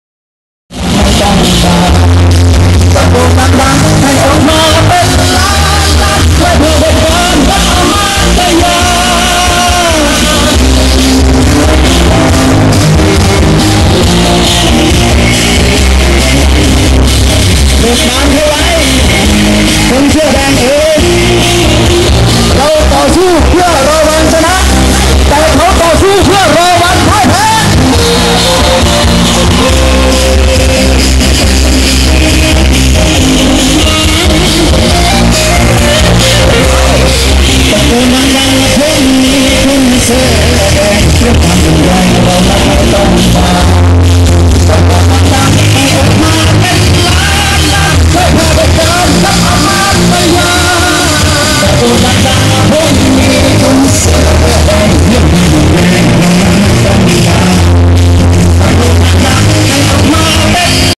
อเล็กซ์หน้ารัฐสภา
บรรยากาศของคนเสื้อแดงกับอเล็กซ์หน้ารัฐสภา ในวันพฤหัสที่ 7 มิ.ย 55